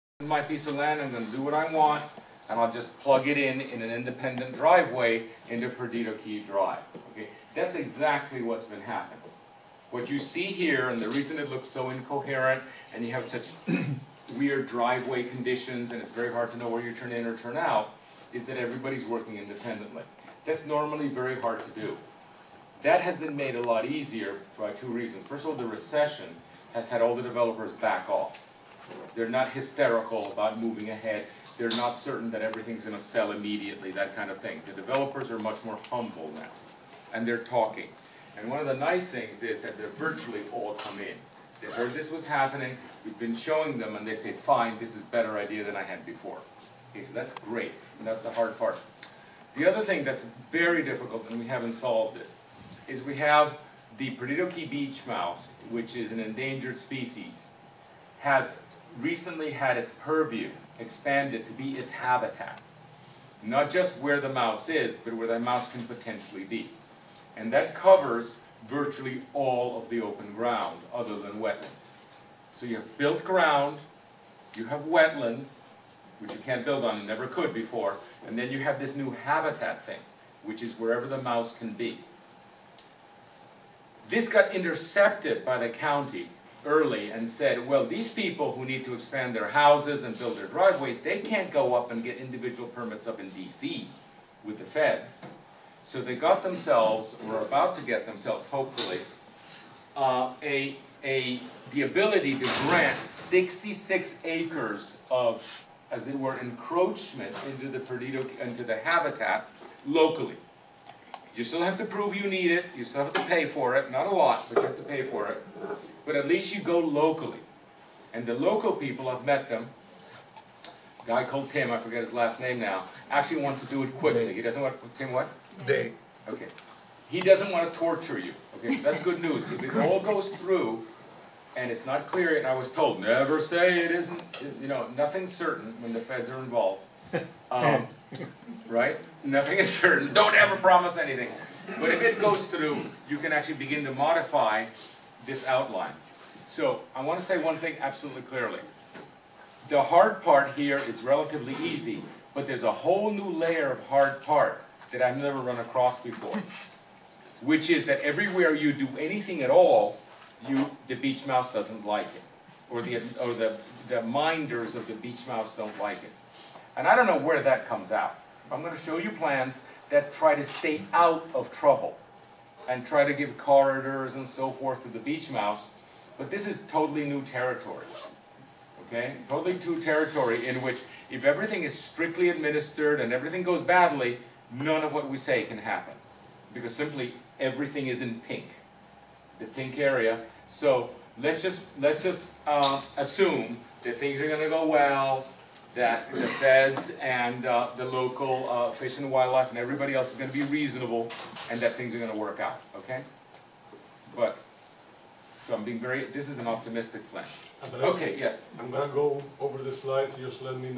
Perdido Key Master Plan Charette Pin-up Meeting 10-18-12 video 2
Perdido Key Master Plan Charette Pin-up Meeting 10-18-12. These videos are the first half of this meeting and will give you a feel of what is being accomplished by the talented group of Consultants. If you are interested in this it will fascinate you. If you are not interested it will probably bore the living daylights out of you.